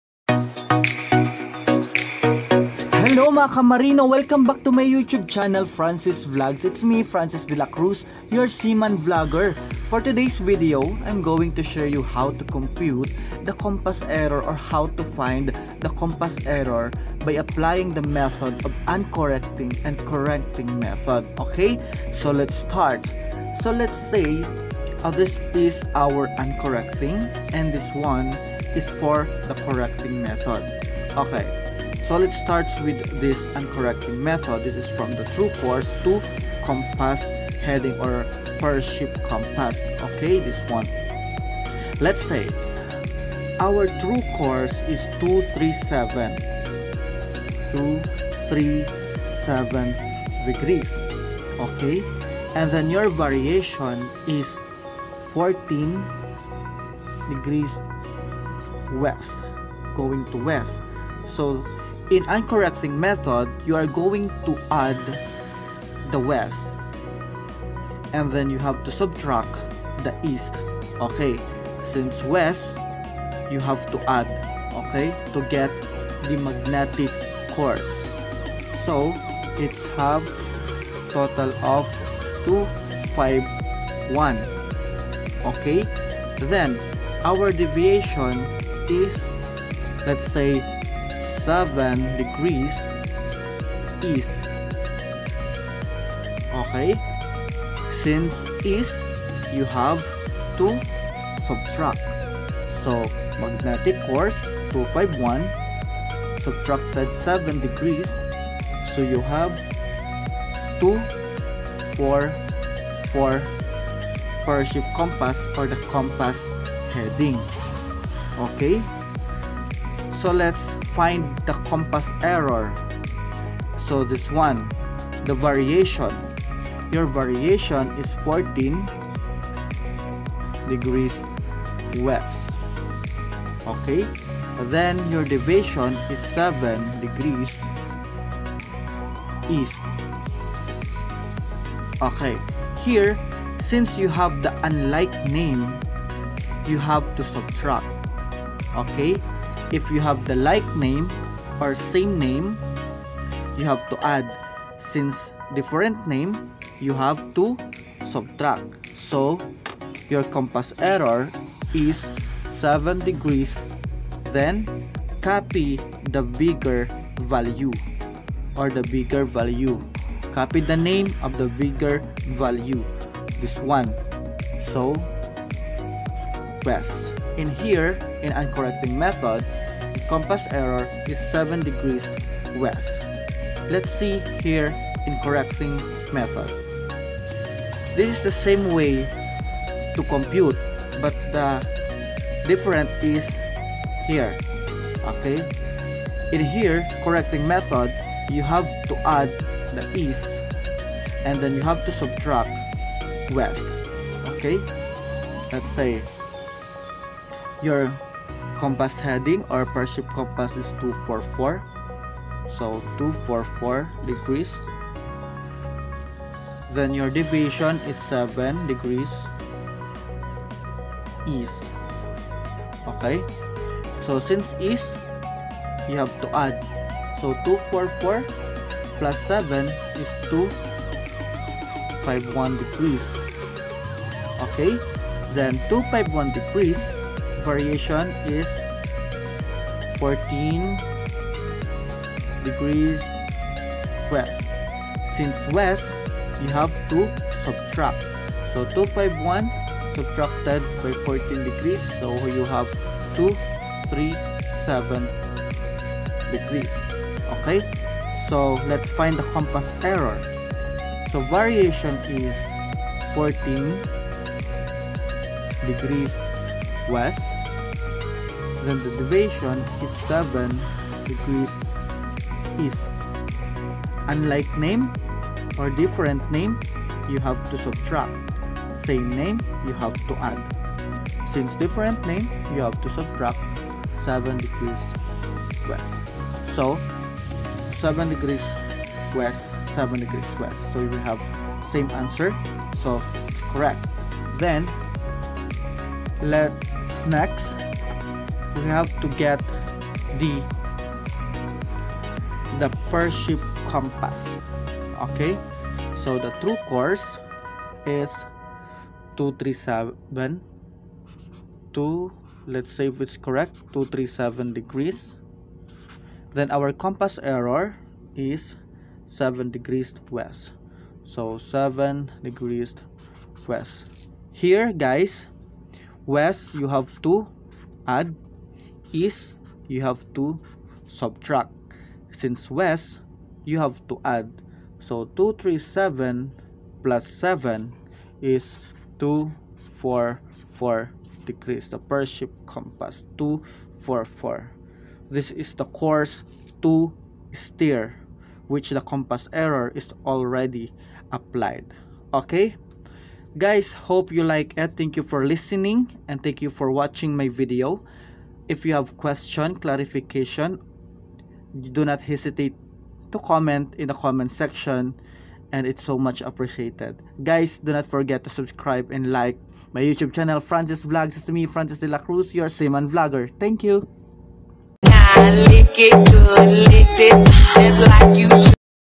FINDING COMPASS ERROR BY CORRECTING AND UNCORRECTING METHOD|NAVIGATION|BY Francis dela cruz
It is necessary for mariners to find or look for a compass error in order to know what course to steer. We must be mindful about the errors of our compasses.